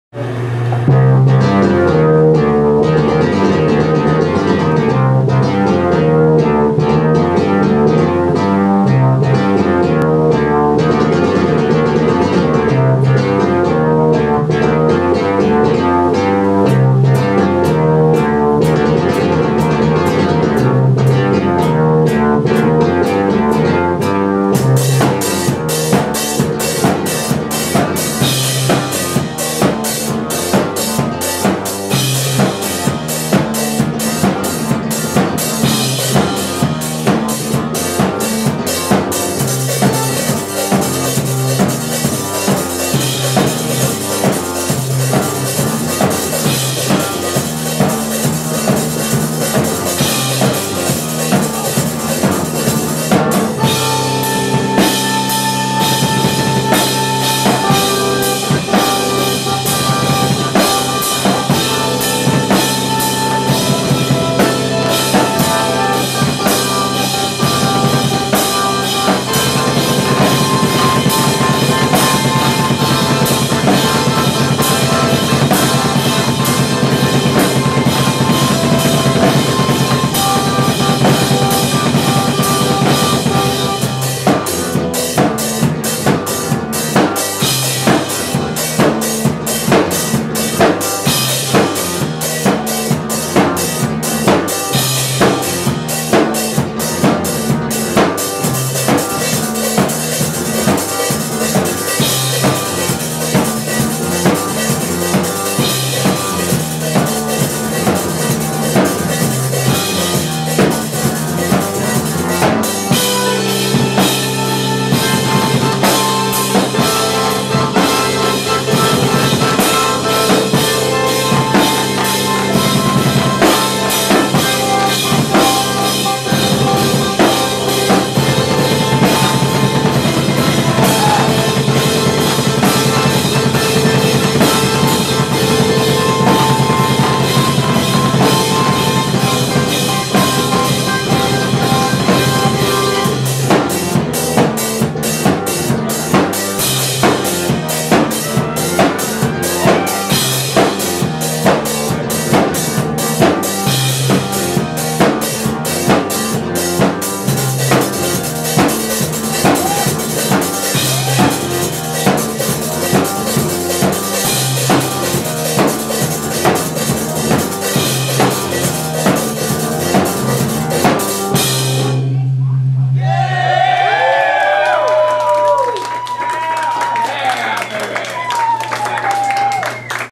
Sich Shifter @ Pehrspace 8/4/08
Dalton and Kyle H. Mabson play at Pehrspace on Sean Carnage's 3rd year anniversary of his monday night shows.

*note*
it's kinda bumpy at some points, so i'm gonna need to upload it again at some point in time.